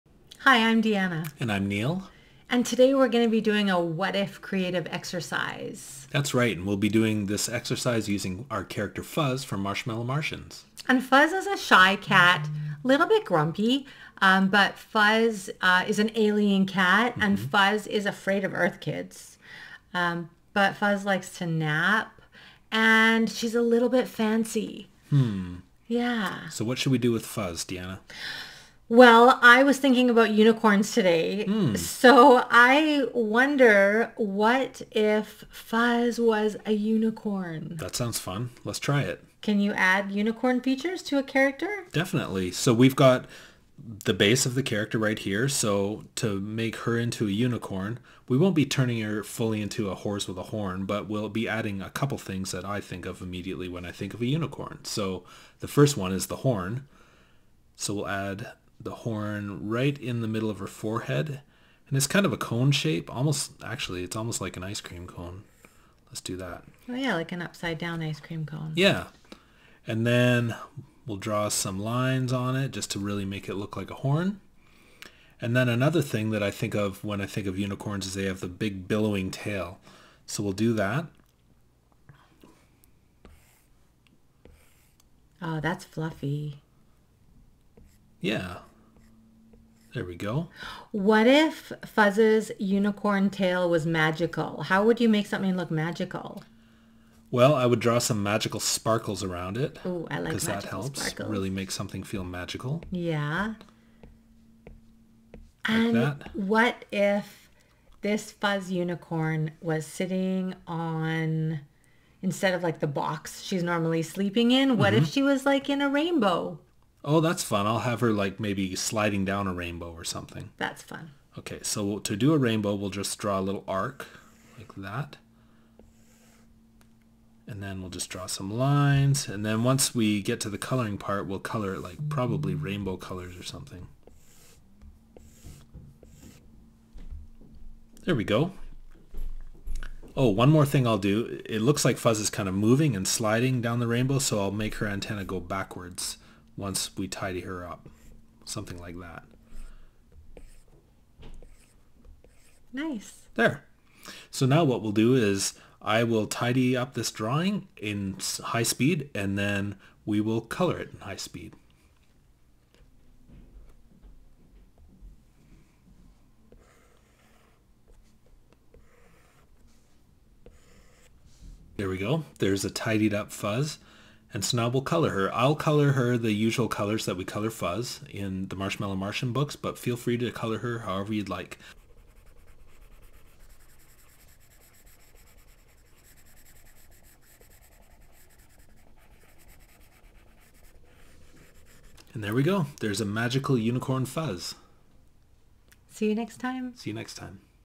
What if… Imagination + Drawing! You can turn a cute cat into a magical unicorn.
Kids! Learn how to draw a cute kitty cat magical unicorn. In this “What if” brainstorm, Deanna and Neil start with a drawing of the cute, squishy Marshmallow Martians cat called Fuzz and get creative! Deanna asks…“What if this cat was a unicorn?”

Neil shows you how to draw a unicorn horn to the cat’s head, turns the cat’s tail into a fluffy unicorn tail and then adds a rainbow and magical sparkle!

Tune in to learn how to draw step-by-step with Neil and to join some creative brainstorming by asking “What if…”

0:00 - Intro
0:38 - Sketching ideas
3:00 - Coloring